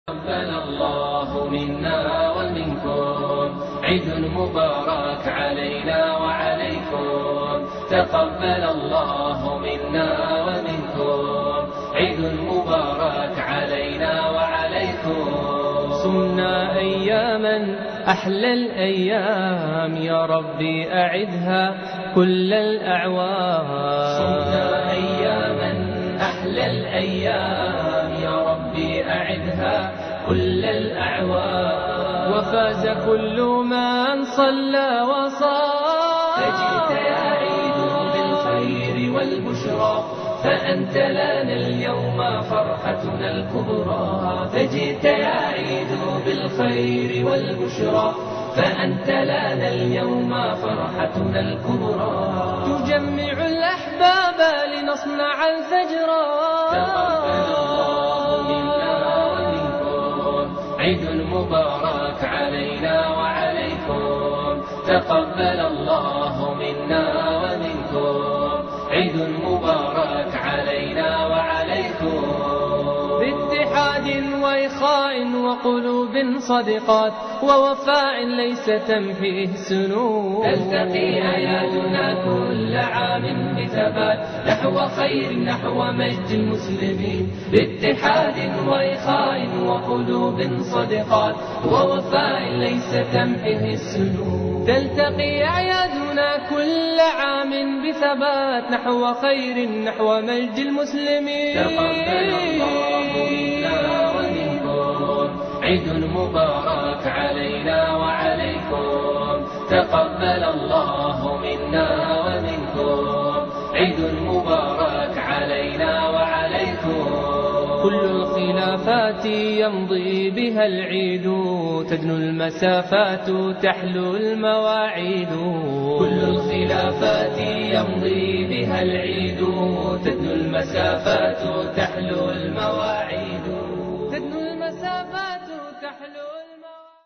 Bridgeview’s Mosque Foundation Celebrates Eid-ul-Fitr 2020
Mosque Foundation's Eid-ul-Fitr 2020 Drive-thru Celebration
Despite the current pandemic, the Mosque Foundation was able to invite joy to the lives of our community members.
Over 3,700 cars (about 10,000 individuals) participated in our Eid celebration while maintaining social distancing. The Masjid leadership greeted families as they drove by our festive campus.